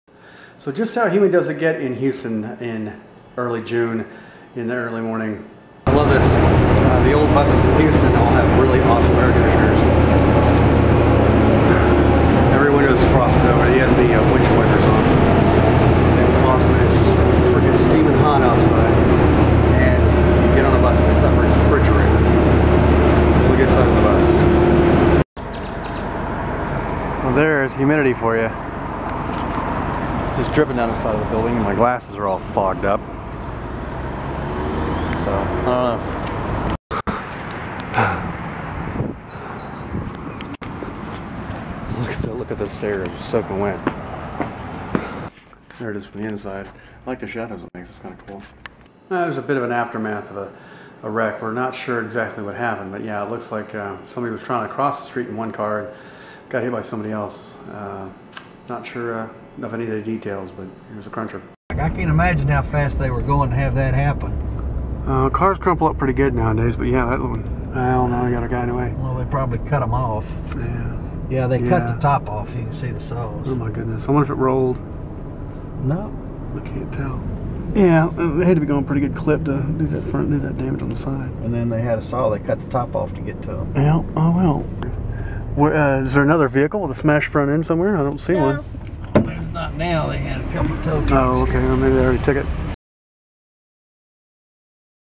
Houston humidity and a car wreck aftermath
It is that time of year again. It is Houston and everything has a sheen of water dripping off it from the humidity. Oh, and we saw the aftermath of a pretty nasty fender bender one the way home.